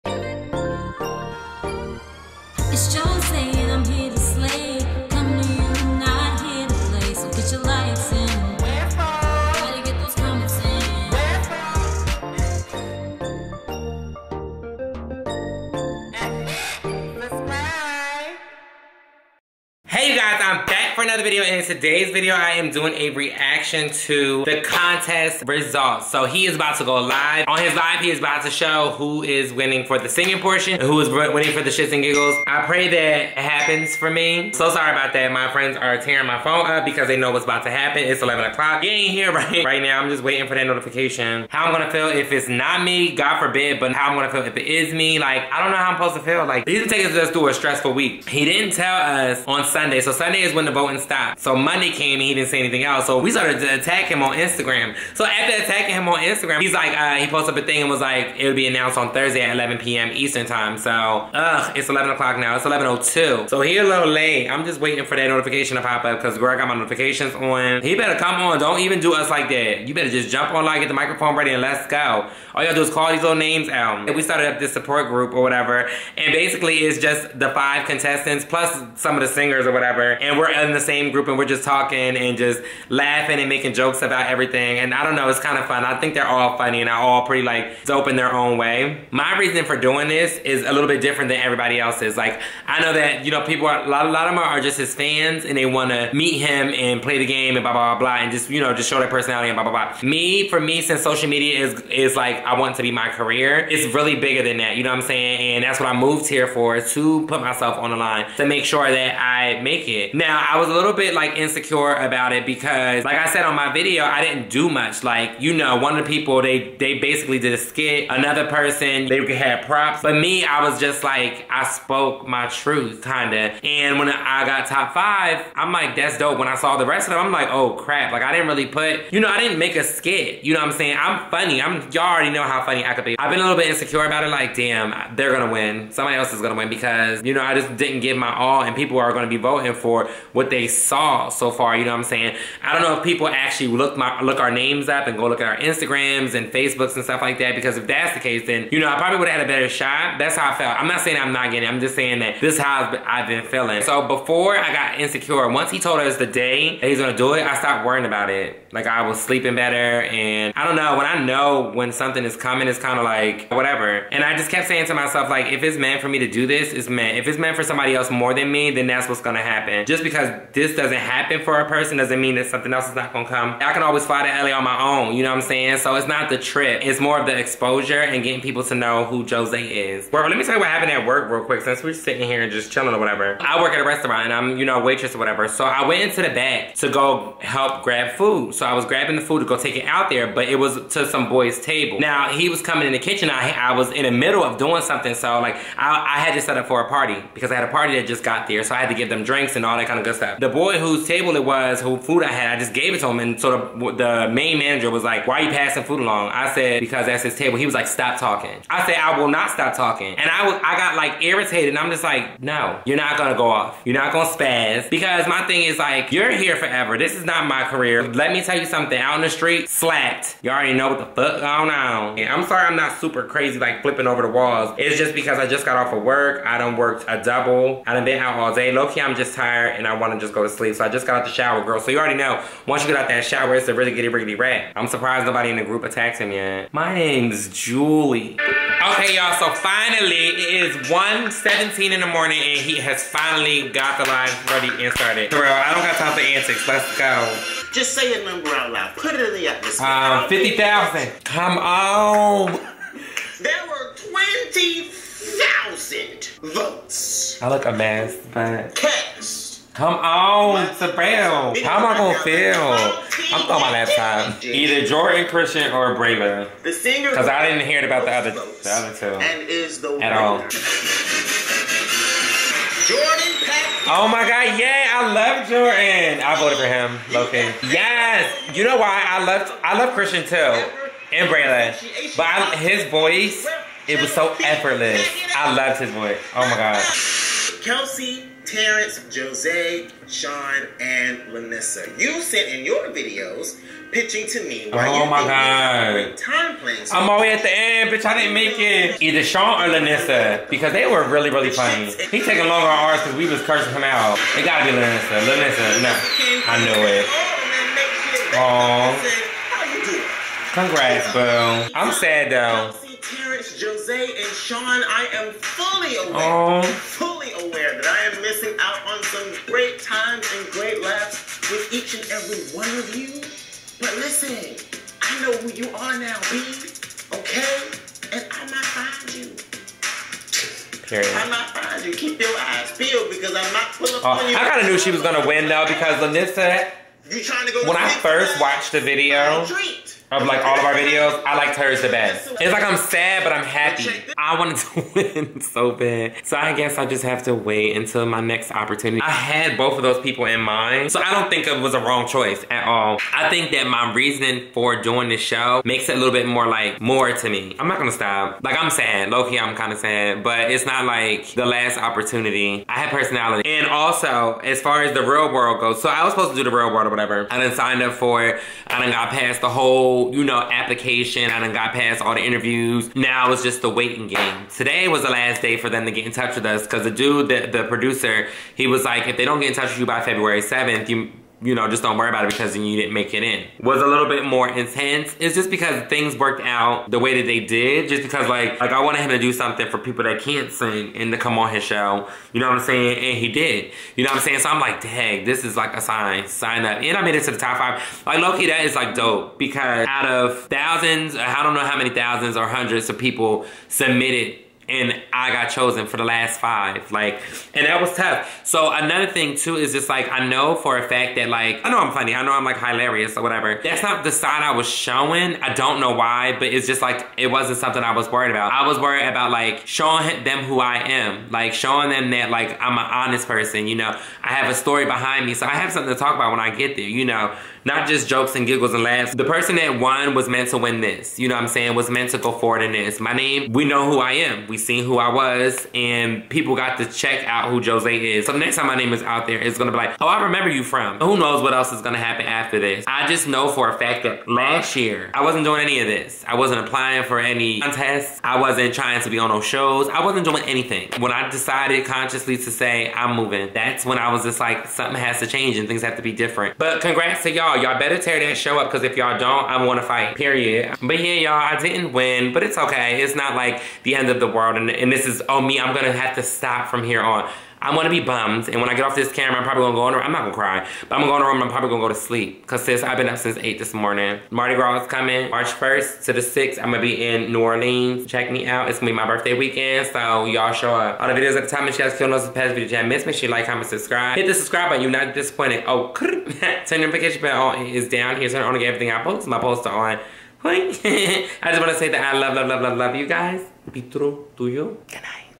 CONTEST RESULTS | JOJO_ROMNEY | REACTION
CHECK TO SEE IF I WON THE CONTEST AND MY REACTION TO IT ! LIVE

Cash.me/$Jhozae - to conveniently send money IF YOU WANT TO -